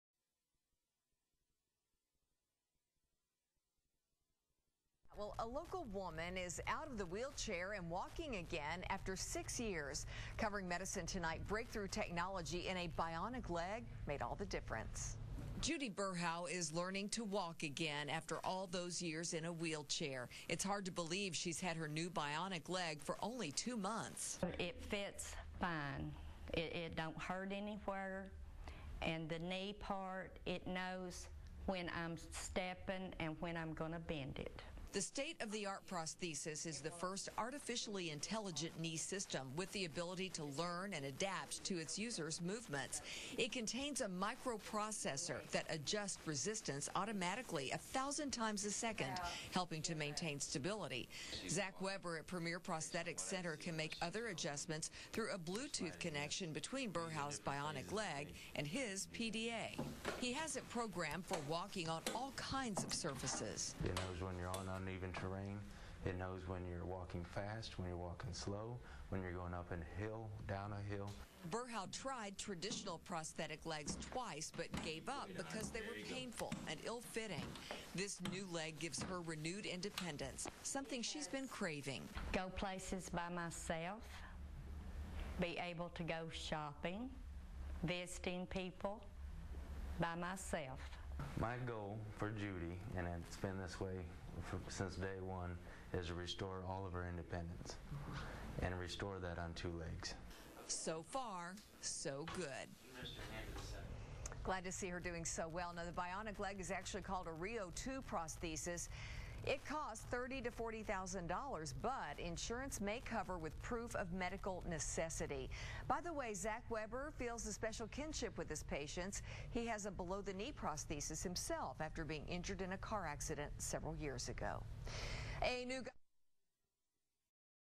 Bionic leg from Premier Prosthetic Center gets Loudon woman walking again
Bionic leg from Premier Prosthetic Center gets Loudon woman walking
again

By Lori Tucker, WATE-TV 6, Knoxville, Tenn.
A Loudon woman is learning to walk again after six years in a wheelchair
thanks to a new bionic leg.

Vascular problems led to the amputation of Judy Berhow's right leg. She
tried two prostheses from different companies with no luck.

She wasn't able to wear either device due to pain.

Recently, Judy learned about the Rheo 2 prosthesis, a state-of-the-art
bionic limb that even has Bluetooth technology.

It made all the difference. Judy was fitted with the new device a little
more than two months ago, and she's already walking.

"It fits fine. It don't hurt anywhere. And the knee part, it knows when
I'm stepping and when I'm going to bend it," Judy said.

It's the first artificially-intelligent knee system with the ability to
learn and adapt to its user's movements.

The limb contains a microprocessor that adjusts resistance automatically
1,000 times a second, helping to maintain stability.

Zach Weber, at Premier Prosthetic Center, can make other adjustments
through a Bluetooth connection between Judy's bionic leg and his PDA. He
has programmed it for walking on all kinds of surfaces.

"It knows when you're on uneven terrain, when you're walking fast, when
you're walking slow, when you're going up a hill, down a hill," Weber
explained.

The new device gives Judy renewed independence. "I can go places by
myself. I'm able to go shopping, visiting people by myself," she said.

The bionic leg costs $30,000 to $40,000, but insurance may cover it with
proof of medical necessity.